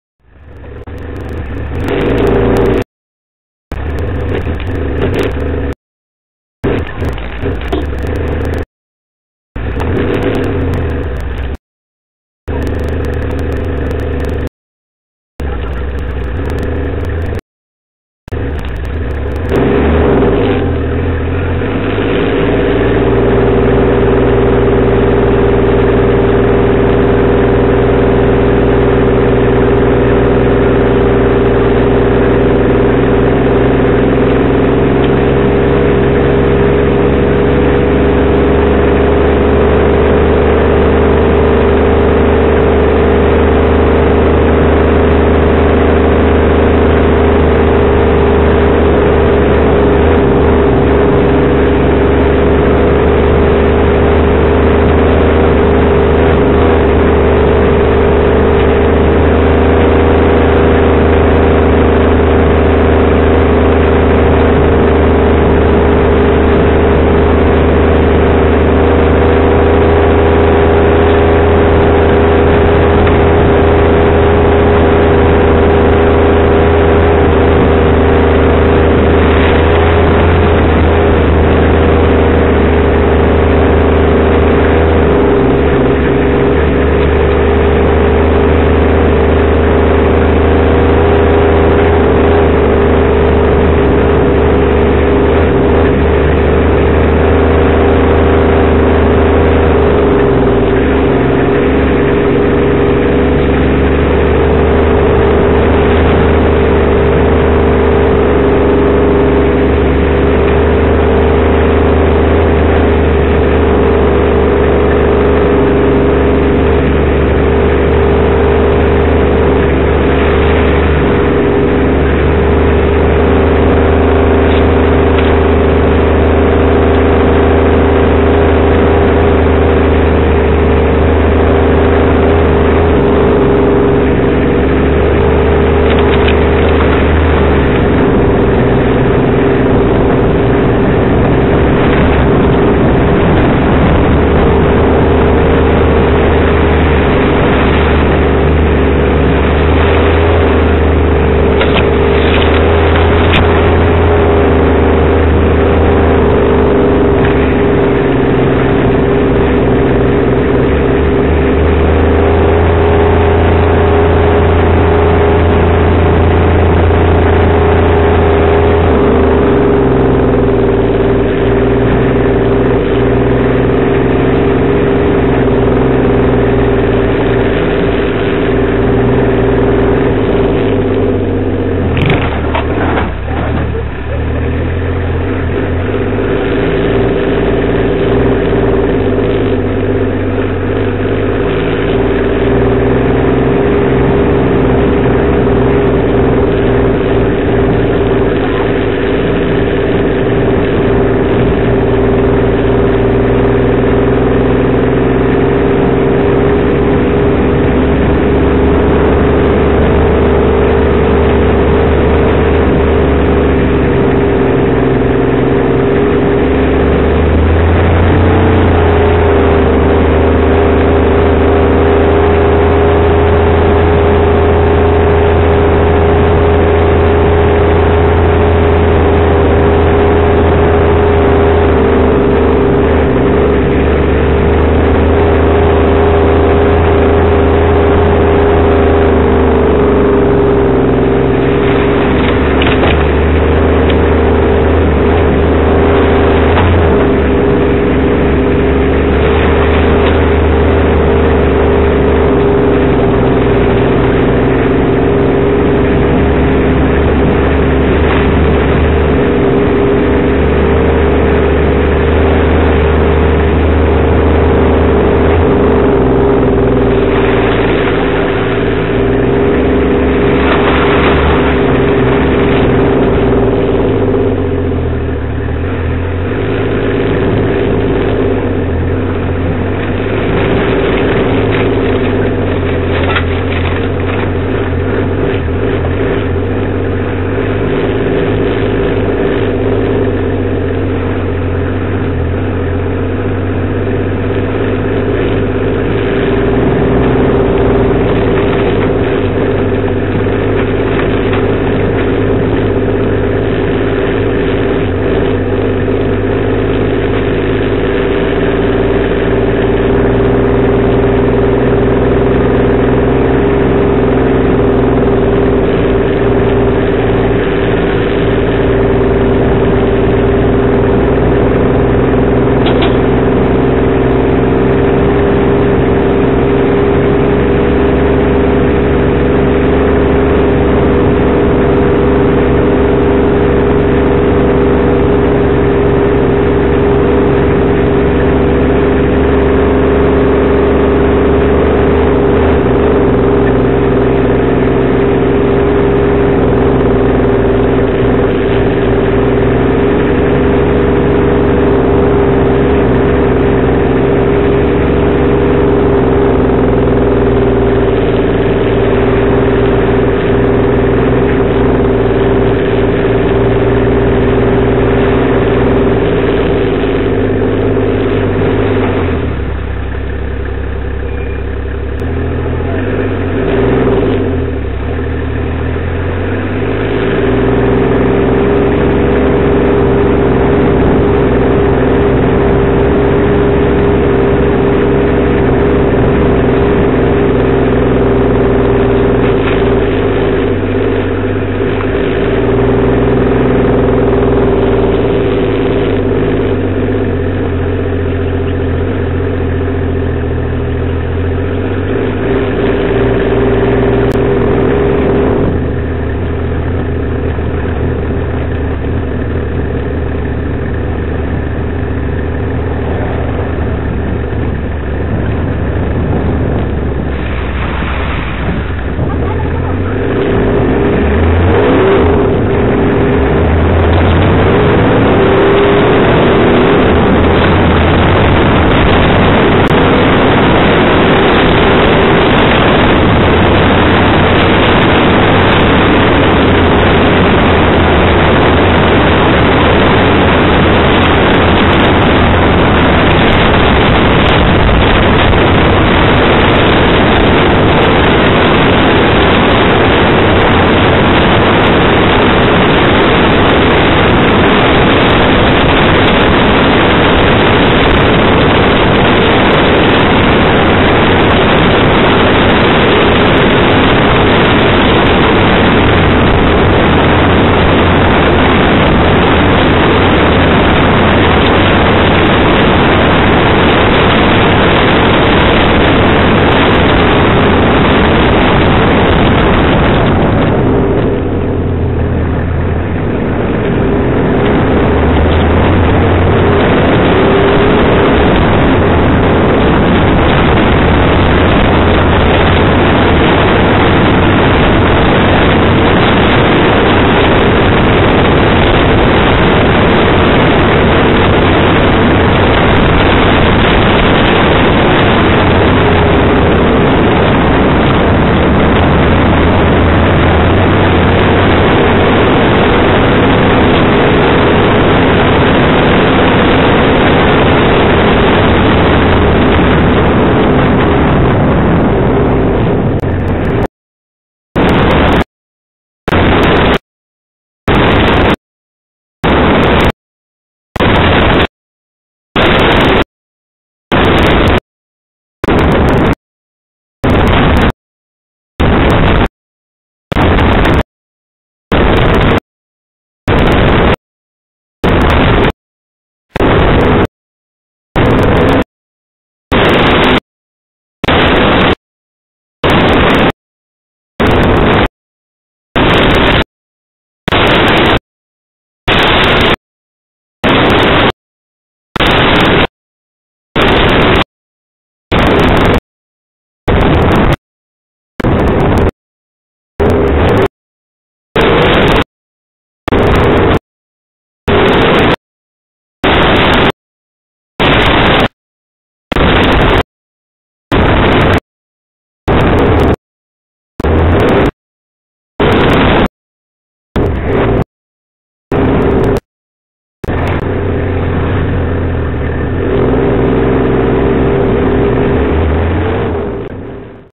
From the Markham River.wmv
A little drive to the bush - swampy, but not to dense jungle really. The four wheel drive comes handy here to avoid bogging.